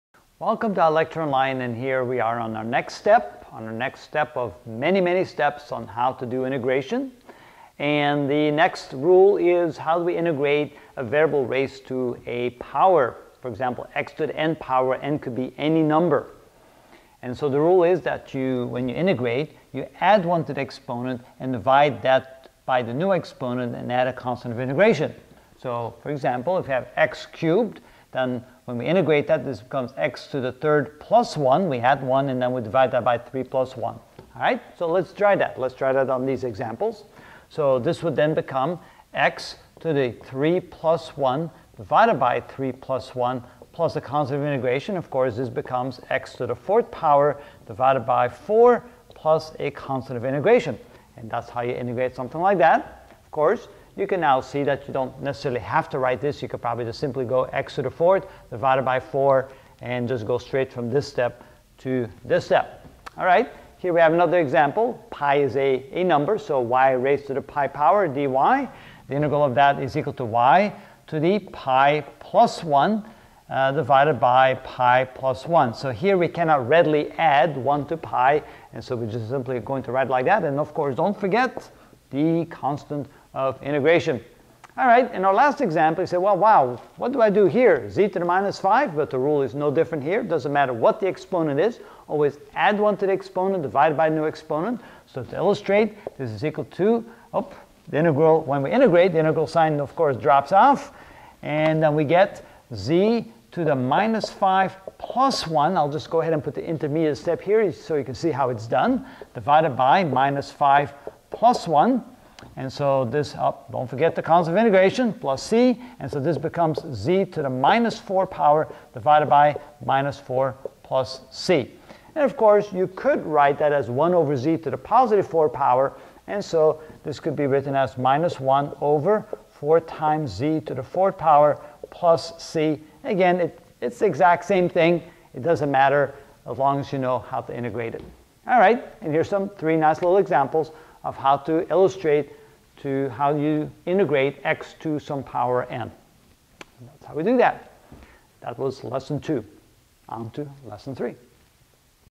Calculus - Integration Basics (2 of 5) Integrating a Variable Raised to a Power
In this video I will show you three examples of integrating a variable raised to a power.